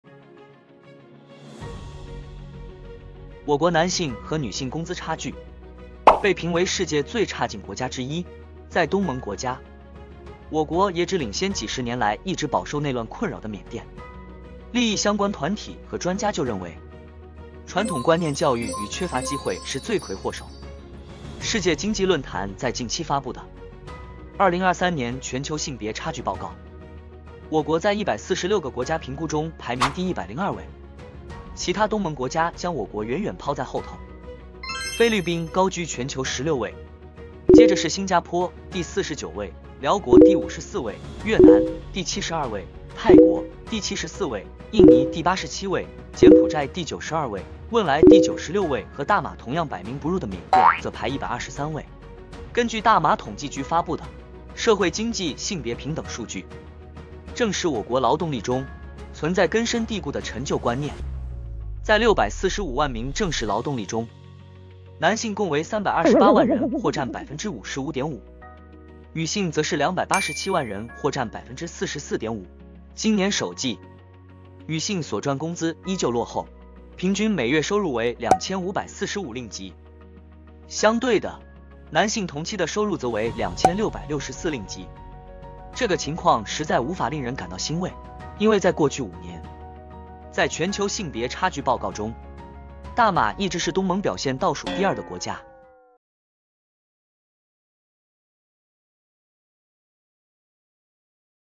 【南视界】只赢缅甸 大马在这方面输完
我国男性和女性工资差距被评为世界最差劲国家之一。在过去5年，大马一直是东盟表现倒数第二的国家。